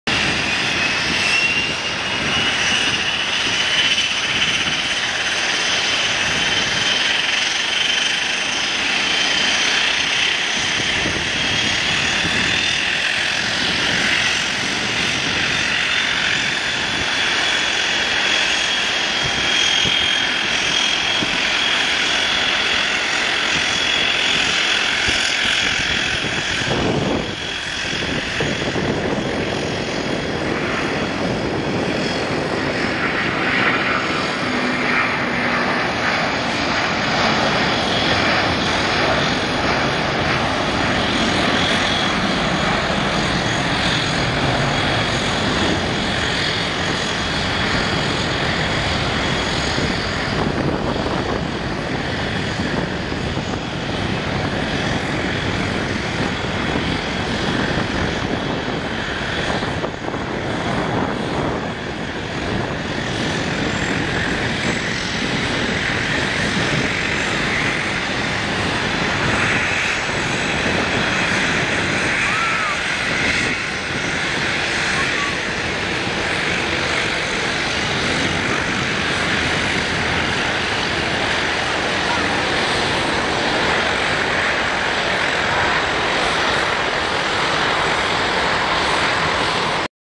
Delta Airlines Airbus A330-200 Takes Off From KPDX On Runway 28R
An Airbus A330-200 for Delta Airlines takes off from Portland International Airport (KPDX) on Runway 28R.